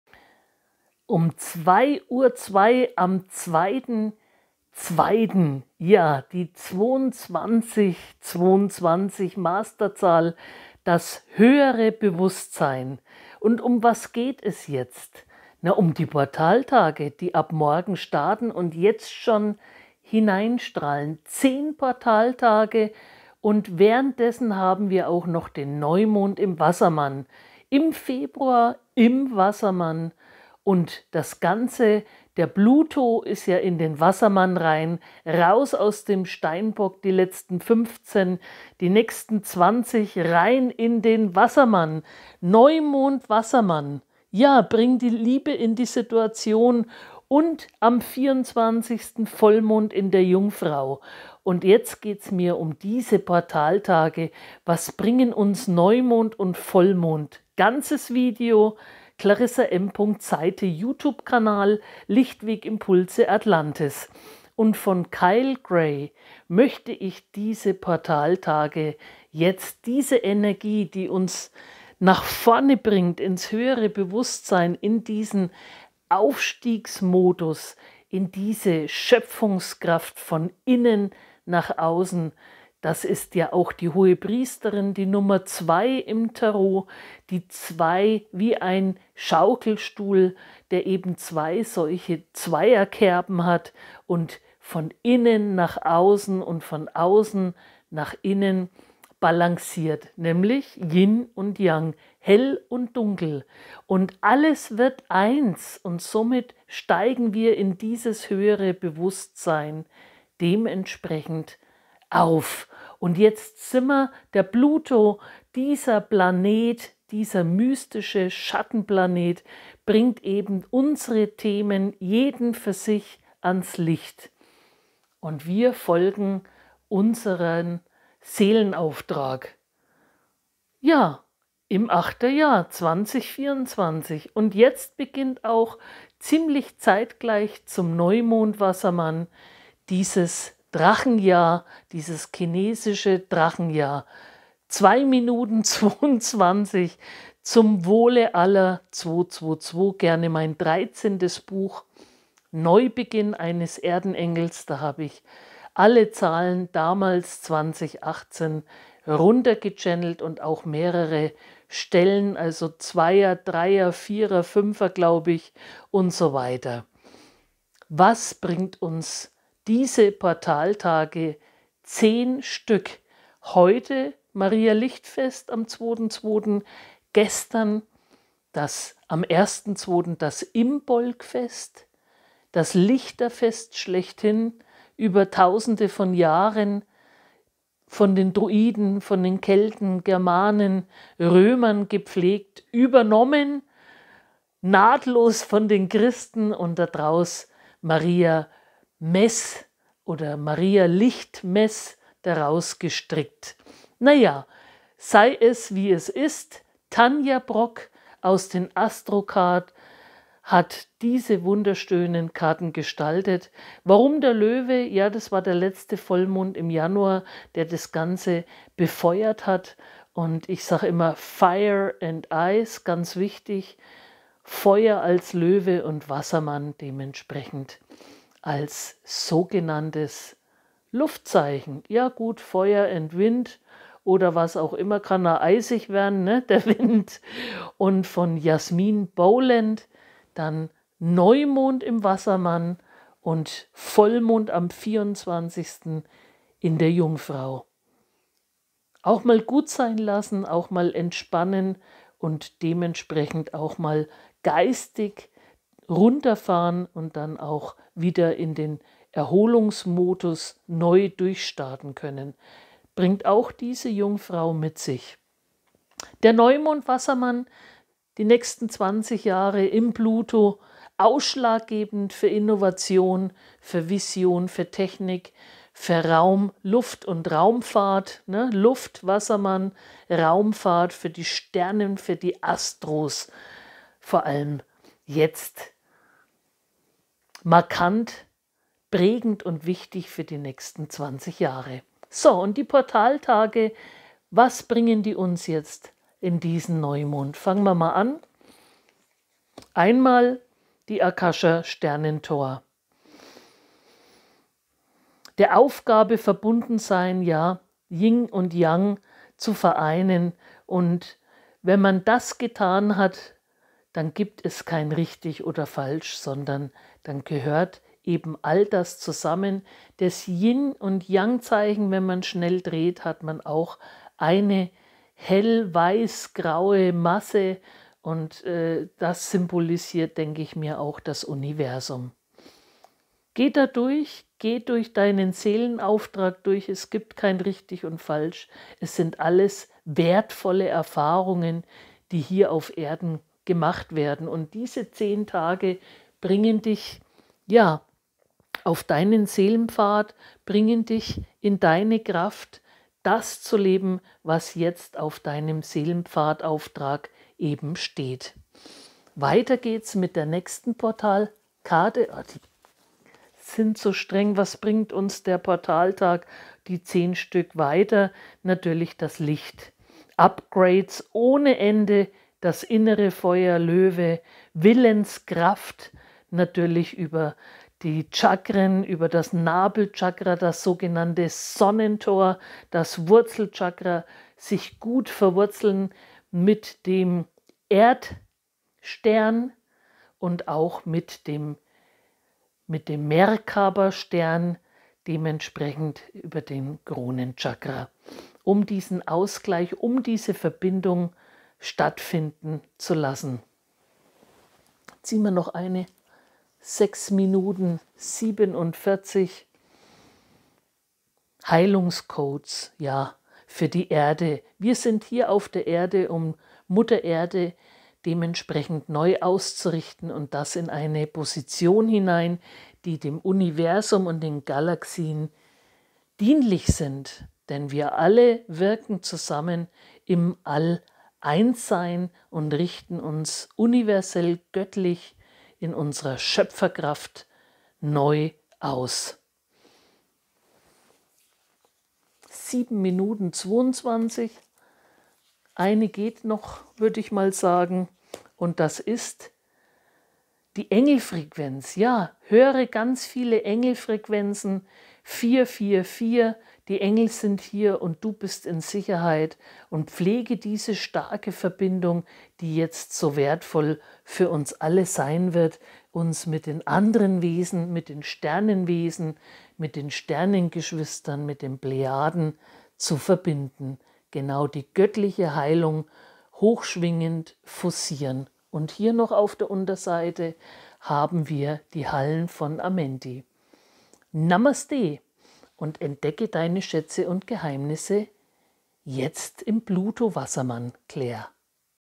10 PortalTage22☆22Neu●♒♍○Mond
°•○●☆22♡22☆●○•°
IMBOLC ☆ LICHT ☆ KOMMT ☆ JETZT

HEUTE
02.02 um 02:02 MariaLicht

DIE ZEIT IST REIF!

10. PORTALTAGE STRAHLEN JETZT SCHON IN DASS LICHT HINEIN

02.02.

NEUMOND WASSERMANN ♒
09.02./11
MEISTERSCHAFT

FEBRUAR IM WASSERMANN
WIRD ASTROLOGISCH GEGENÜBERLIEGEND WEITERHIN DURCH DEN
VOLLMOND 🦁
°•○●☆333☆●○•°
BEFEUER
YES!!!
JETZT BEGINNT EINE WILDE FEURIGE ZEIT
PLUTO IM WASSERMANN ♒
SPRICH

FEUER & EIS
FIRE & EIS

WARM anziehen?
ODER
sich befreien

PHÖNIXKRAFT
DRACHENKRAFT

2024/8ERJAHR
DRACHENJAHR HOLZ

WIR WERDEN ES SEHEN, SPÜREN ERFAHREN...
WO UNS DER WASSERMANN ♒
UNS IN DEN NÄCHSTEN 20 JAHREN
HINFÜHREN WIRD

DIE KRAFT DES ♌ LÖWENS
JETZT IM VOLLMOND
UND
DER DRACHEN
FÜHREN UNS KRAFTVOLL
EIGENVERANTWORTLICHT
MACHTVOLL
AN

WIR WERDEN UNS IMMER BEWUSSTER
WER WUR SIND
WO WIR HERKOMMEN
WAS UNSER WIRKEN
UNSER AUFTRAG
IST

GedankenBlitze, die sich im Februar in den Portaltagen zeigen möchten.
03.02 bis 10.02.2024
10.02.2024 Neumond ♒
Über kurze Visionen im inneren Auge oder
Gedanken, die sich Dir zeigen und dir einen Einblick in deinen weiteren Verlauf deines Seelenpfades ermöglichen! An diesen Portaltagen haben wir einen weiteren besonderen Schlüssel, dass
 IMBOLC LICHTERFEST ALS AUFTAKT AM 01. FEBRUAR 2024!!
°•○●★21▪︎12★●○•°
Für mich ein grosser
33
,der diesmal die PortaltagReige von
8 Tagen //8ERJAHR
Jetzt im Februar im Wassermann
besonders erÖffnet!

Dazu auch gerne meine Videos

CLARISSA M. SEITE
LICHTWEGIMPULSE ATLANTIS

vor allem
Pluto, der nach 15 Jahren aus dem
♑ Steinbock kommt
Und jetzt seit dem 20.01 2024 nun in den Wassermann ♒ für genau 20 Jahre wechselt

Jetzt mit dem FeuerZeichen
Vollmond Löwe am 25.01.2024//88

Haben wir die
KRAFT UND DEN MUT
Pluto in den Wassermann, was uns genau diese
Visionen Gedankenblitze an die Hand geben möchte, um eben auf deinem Seelenpfad vollendst
Dich und deine Vision
und
Deinen Auftrag hier auf Erden, Mensch geworden,  zu erfüllen!

ASTROLOGISCH LIEGEN
LÖWE🦁 WASSERMANN♒
GEGENÜBER
SCHAUEN SICH DIREKT IN DIE AUGEN

KartenQuelle intuitiv heute morgen
und dann für @all gezogen.
KARTENQUELLE
GEZOGEN

Pluto
Dass Universelle Göttliche schiebt einen immer mehr in seine Selbst 222☆Verantwortung☆222
Hinein
Was ist deine persönliche Bestimmung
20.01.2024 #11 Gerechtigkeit
Der Portaltag am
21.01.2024
°•○●☆21▪︎12☆●○•°
PLUTO WASSERMANN
°••●☆2112☆●○•°
NEUE WELT DES VOLKES
DURCH◆◇◆BRUCH
ALTE KONVENTIONEN
GESETZE WERDEN NICHTIG
VollMond 🦁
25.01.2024
•°○●★777★●○°•
°•○●★888★●○•°
Hinweis LöwenTor
08.08. 8ERJAHR
°•○●★888★●○•°
SHIFT
PLUTO WASSERMANN
WIR BRECHEN AUF...
UM NEUES AUF DEN WEG ZU BRINGEN
NEUE WIRK LICHTE
FREIHEIT
UNABHÄNGIG STARK
KRAFTVOLL
MACHTVOLL
EIGENVERANTWORTLICHT
AUF EIN GUTES NEUES
2024/8ERJAHR
Vergangene Träume, die oft geträumt wurden,
 dürfen sich wie der Phoenix
 selbst verbrennen, um dann in einem neuen Gewand, farbenfroh,
 voller Glitzer und Glimmer
 in all seiner Kraft neu geboren in dir entstehen darf.
 entfaltet, im 8 Jahr der Eiche, voller Kraft macht Lust die acht im Tarot.
So dürfen wir uns nun neu entfalten, wie eine Geburt, ein Shift, ein großes TOR, ein Achter Tor, was sich am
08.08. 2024 in der 8 zum Wohle Aller  ☆○☆☆○☆○○☆○
 Pure Kraft und Wahrheit verwandelt!
☆○☆○☆888☆○☆○☆
JETZT ERHEBT SICH DER PHÖNIX AUS DER ASCHE
EINDRÜCKE DER MÄCHTIGEN ENERGIEN
ÜBER DIE FESTTAGE DER HEILIGEN ZEIT
☆JESU GEBURT☆
ERNEUERUNG
★ALLES IST MÖG☆LICHT★
Alle PortalTage
FEBRUAR 2024:
Samstag, 3. Februar 2024.
Sonntag, 4. Februar 2024.
Montag, 5. Februar 2024.
Dienstag, 6. Februar 2024.
Mittwoch, 7. Februar 2024.
Donnerstag, 8. Februar 2024.
Freitag, 9. Februar 2024.
Neumond ♒ 🧜‍♀️  23:58 MEZ
Samstag, 10. Februar 2024
Als Abschluss Portaltage mit dem Neumond
Wassermann im Wassermann ♒ im Februar
Der 2HohenPriesterin Doppelte Komponente
zugeordnet
Vollmond ♍ ♍ 13:31 MEZ
Dienstag, 24. Februar 2024

☕ TRINKGELD ☕
CLARISSA M. SEITE
HEILPRAKTIKERIN FÜR PSYCHOTHERAPIE
[HEILPRG]
SUCHTBERATERIN
REIKI 🌟  MEISTERIN/LEHRERIN SCHREIB-UND SPRECH
MEDIUM 🌟
CHANNELING
☆°•○●★888★●○•°☆